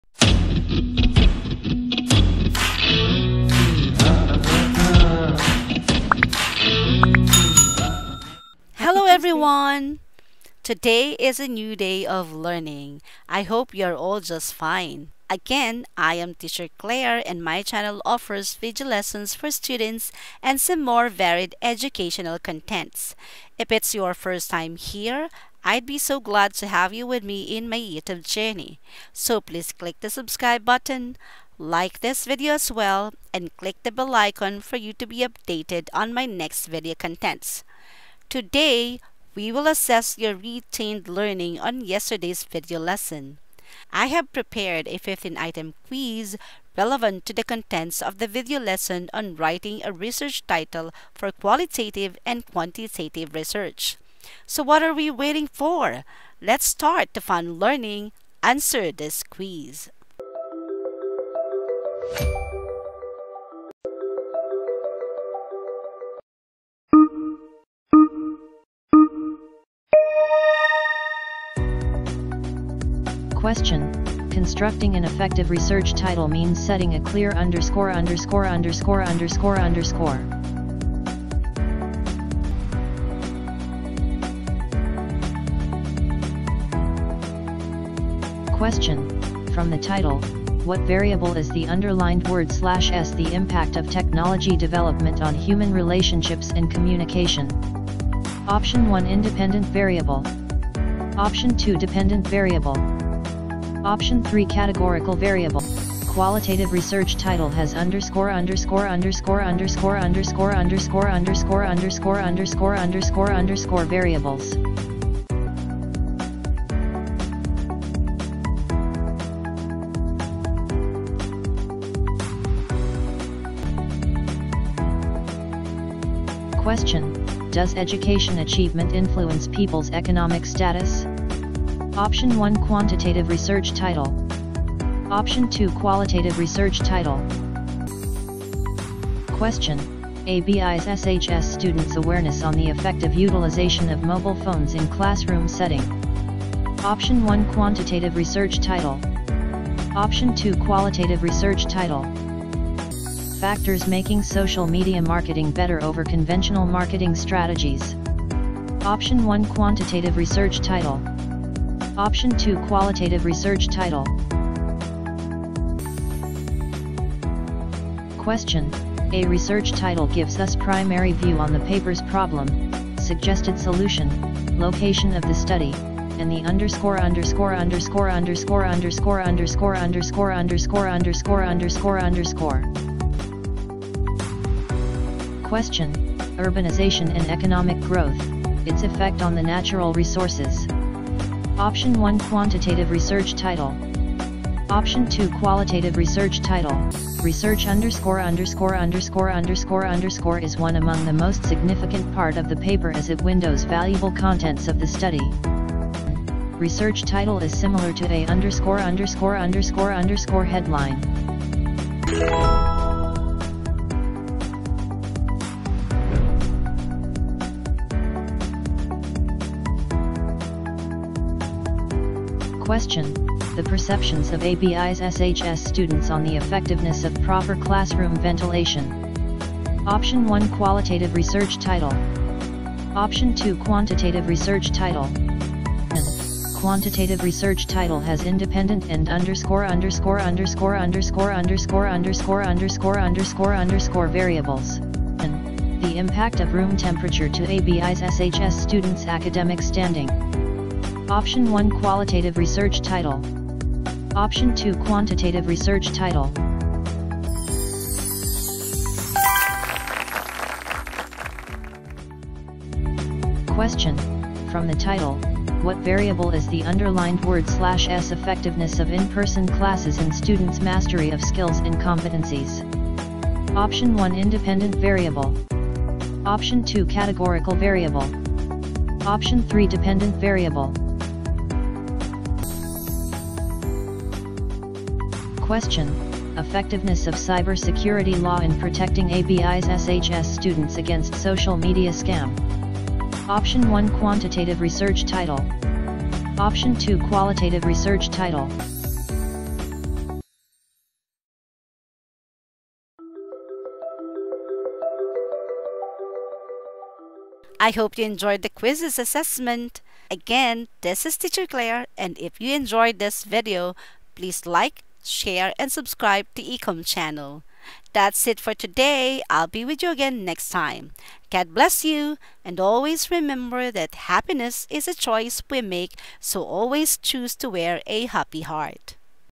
QUANTITATIVE vs QUALITATIVE RESEARCH TITLE
To test your knowledge on the distinct differences between quantitative and qualitative research title, watch and answer this 15-item quiz.

Timecodes:
0:00 Intro
1:00 Quizizz

Thank you and God Bless us all! Stay safe and healthy. Enjoy watching and never forget to always wear a HAPPY HEART! ❤️🥰